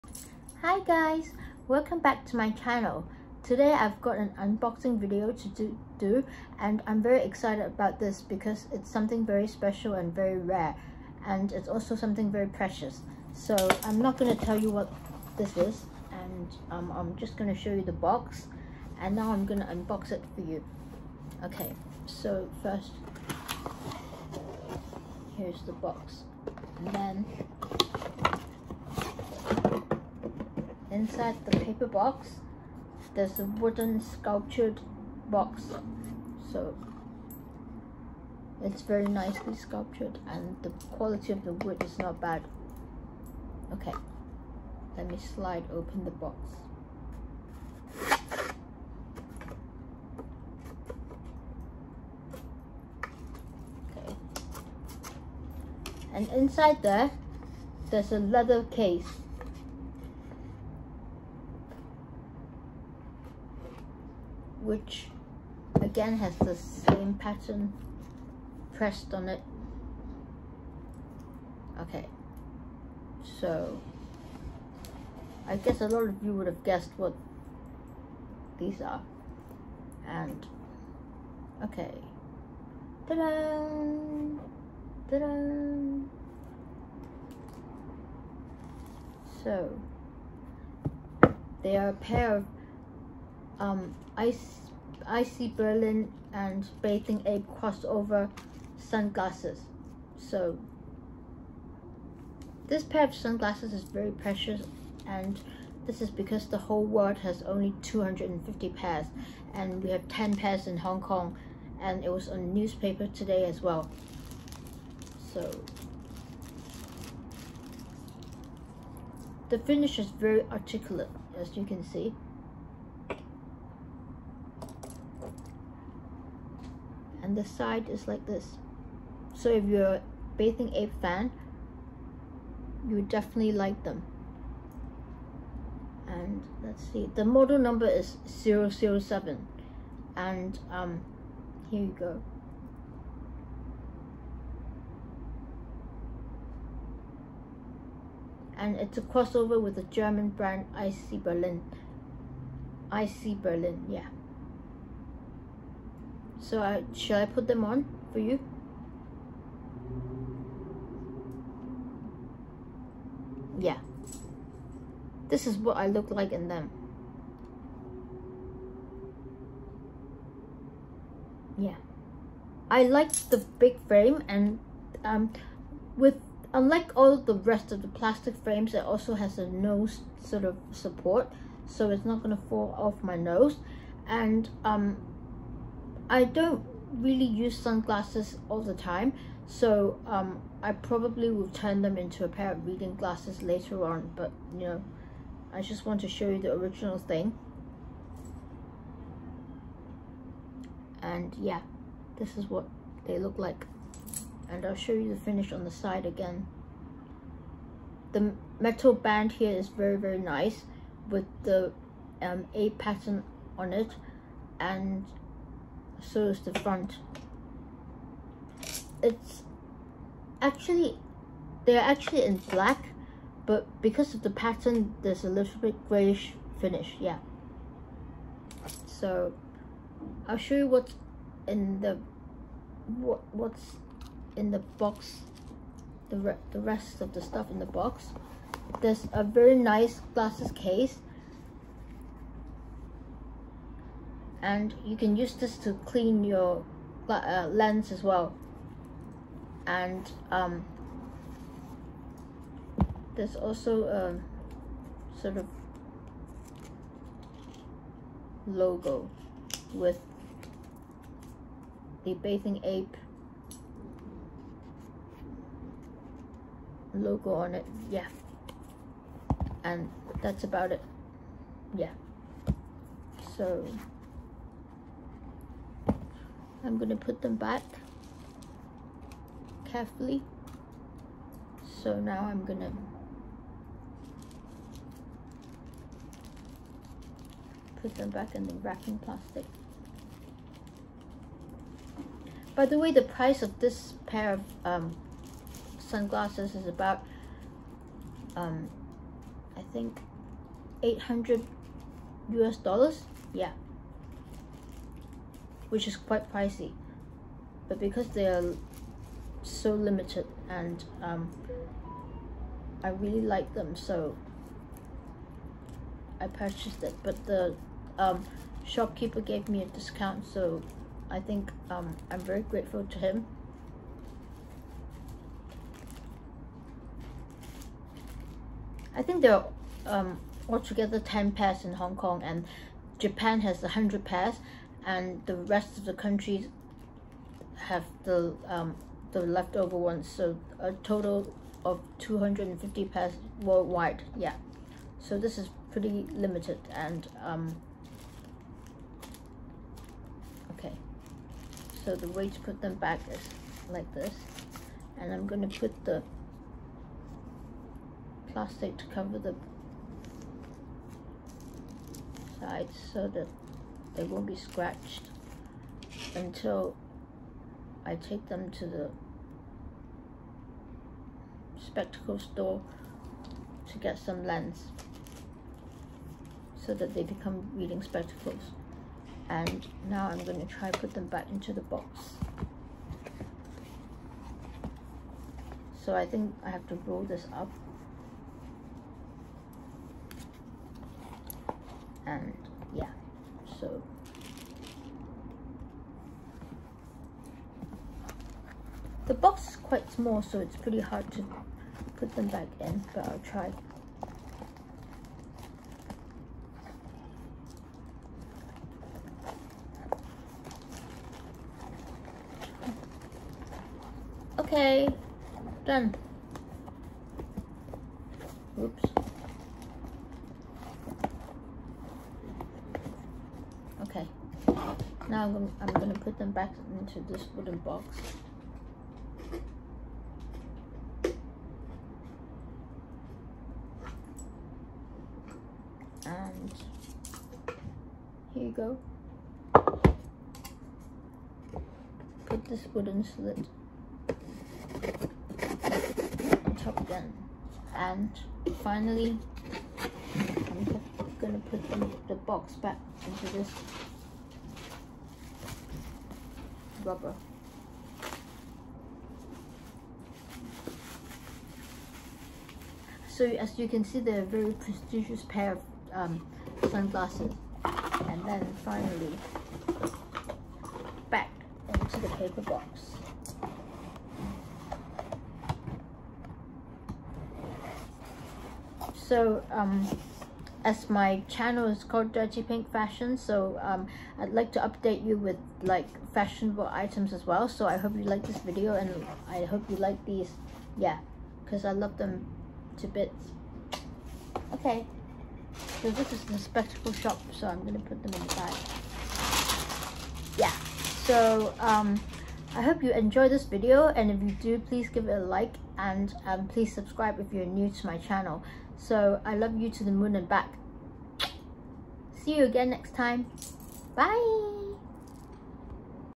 Unboxing and a short review of the ic! Berlin crossover Bathing Ape Sunglasses
A limited edition pair of sunglassesand the whole world only has 250 pairs.  They are pricey but worth it and I really am in love with them.  They are.on the Hong Kong Newspaper today as well.  Thank you Lord I'm lucky enough to have them.